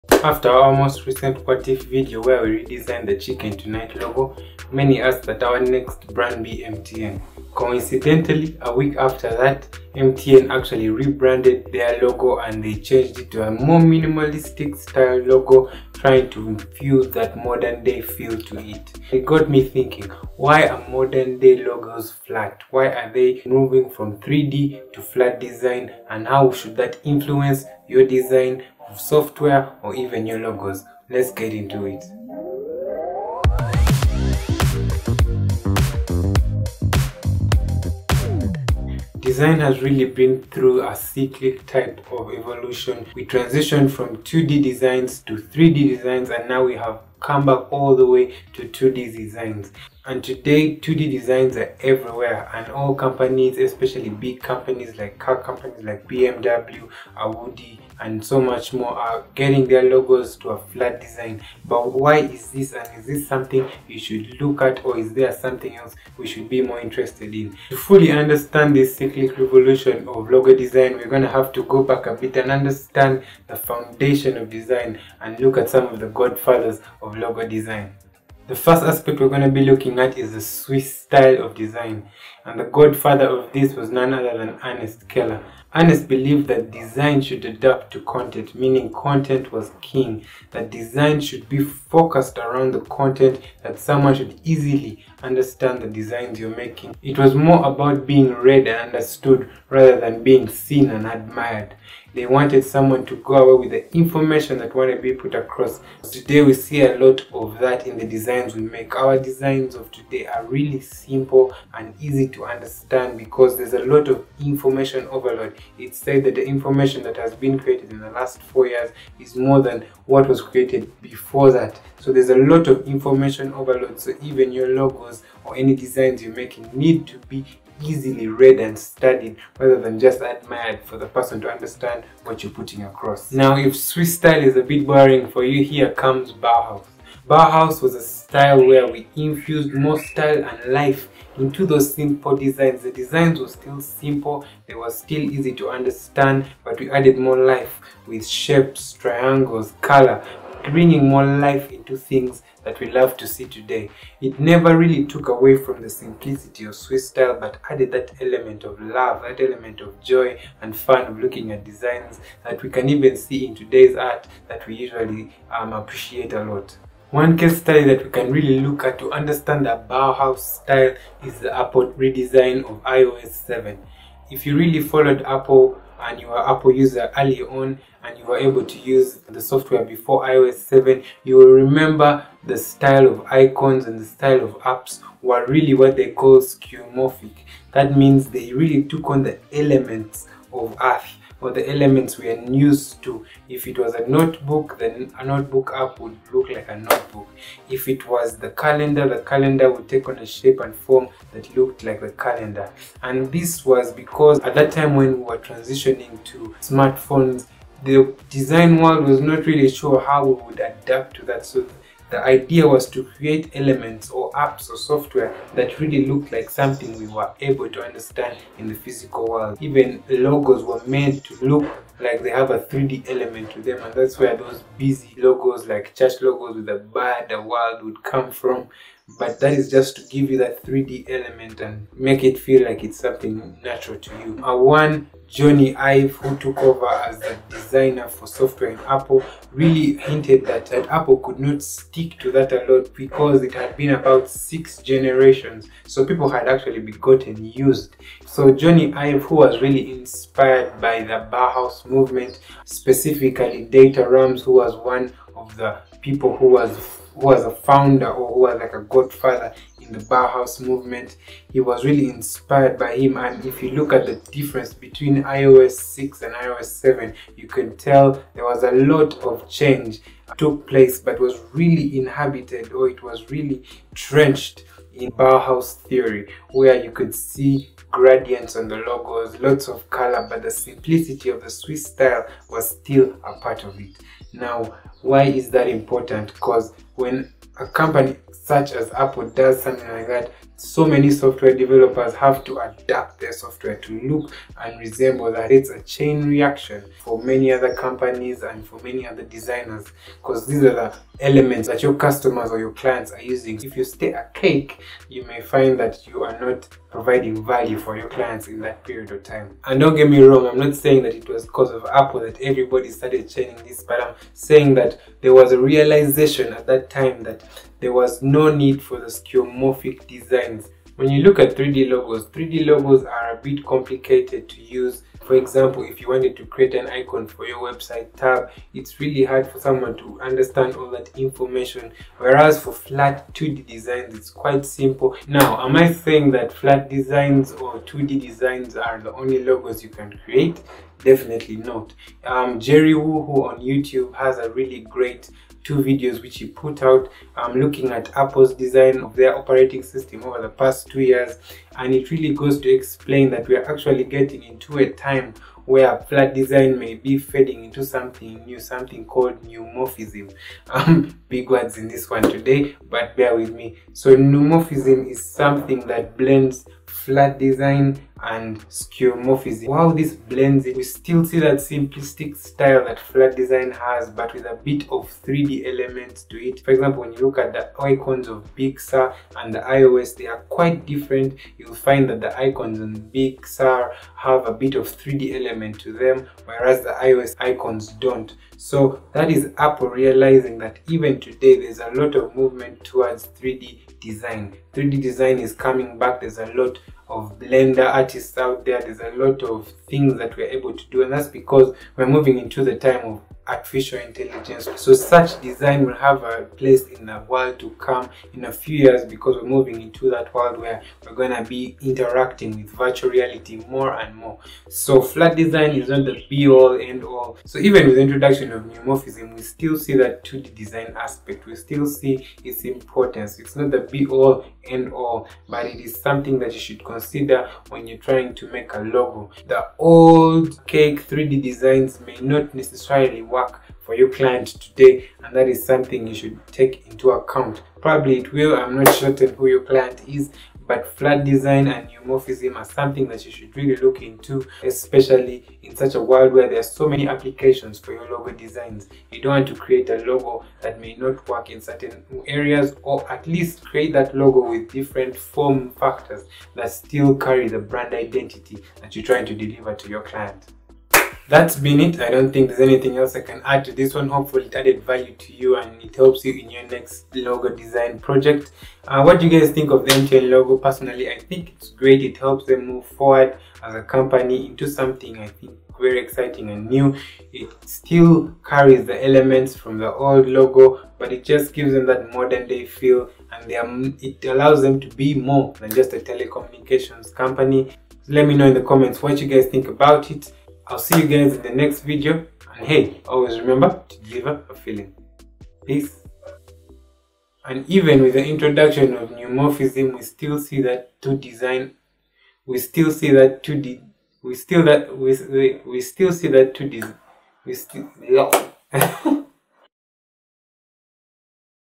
Why Modern Logos are FLAT! | Design Essay
After our most recent What If video where we rebranded the Chicken Tonight Logo, you asked that we redesign the MTN logo. However, MTN unveiled a new identity a week later. But that redesign got me thinking, why are modern day logos falt?

Camera: Canon 6D -
Microphone: MOVO Shotgun mic
Light: Two Solar powered lights
Editing: Premiere Pro 2021